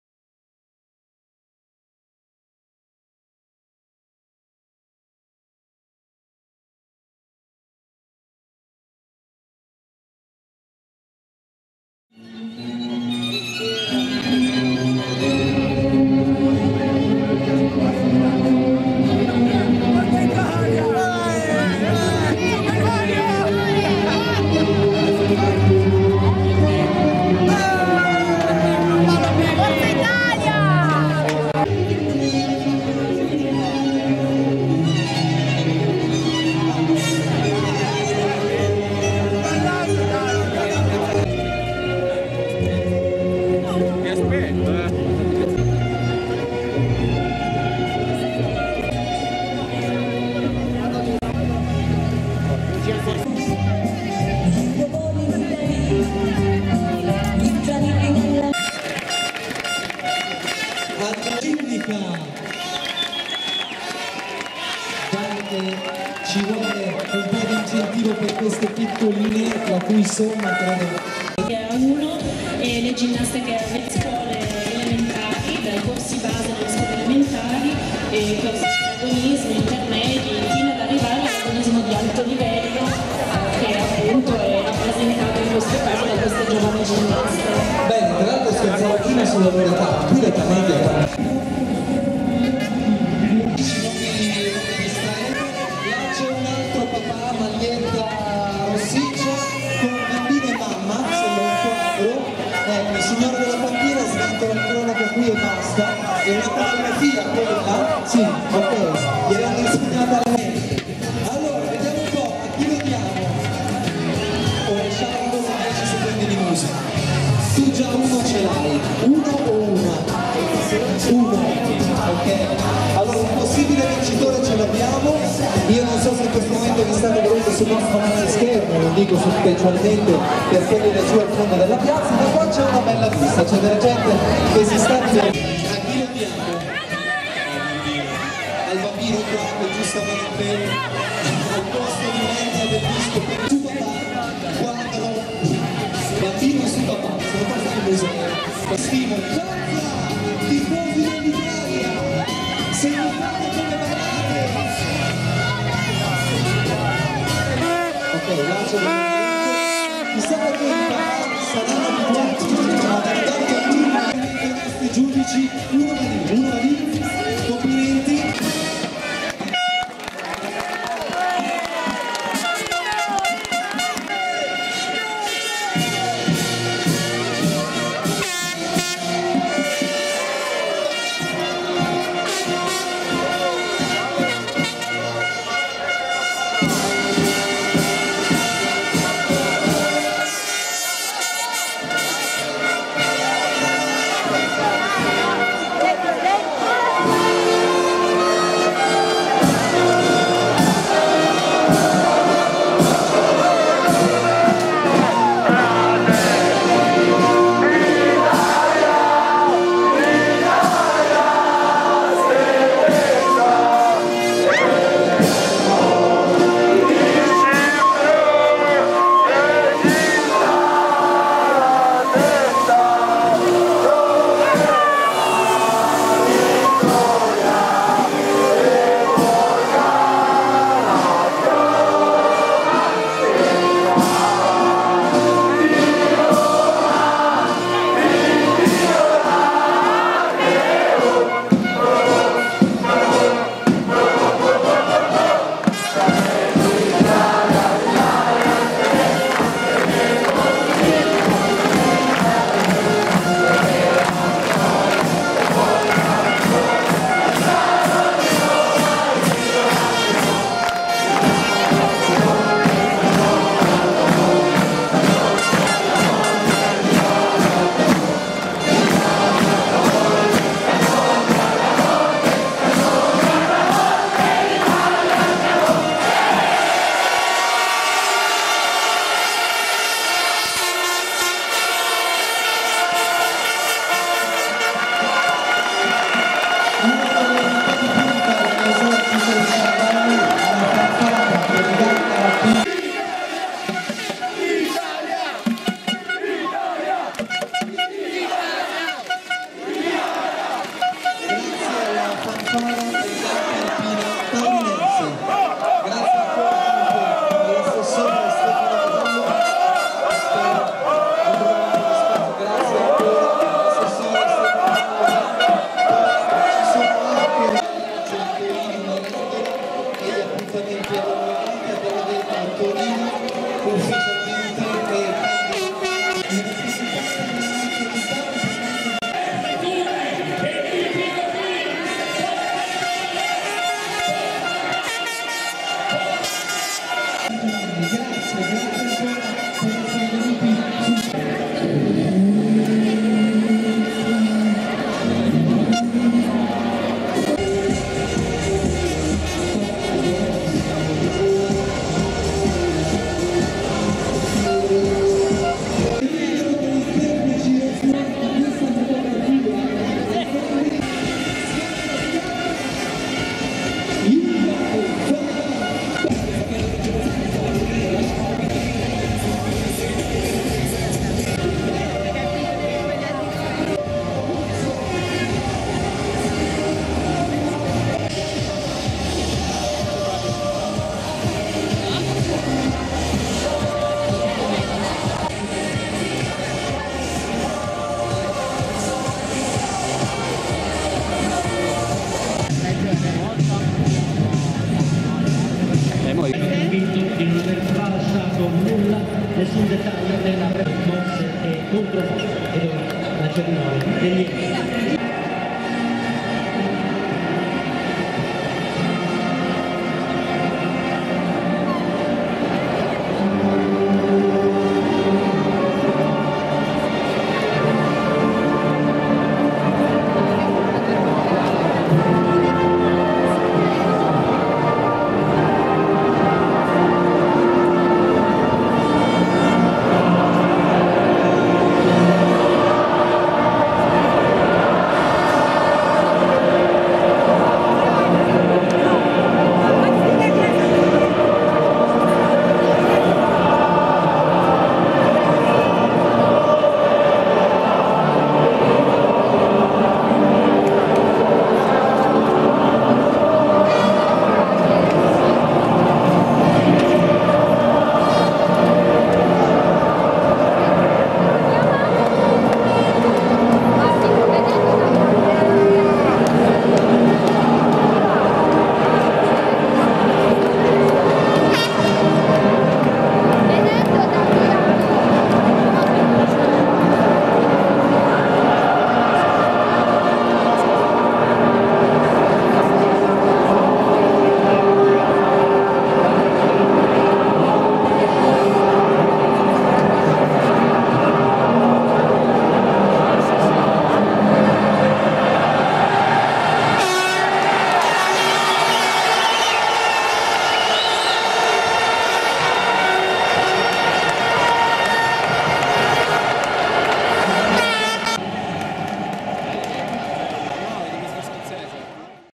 Italia-Inghilterra. 24 Giugno 2012.Torino Piazza Vittorio
Il video è stato realizzato a Torino in Piazza Vittorio in occasione della partita. Documenta l'entusiasmo dei tifosi prima dell'inizio dell'incontro.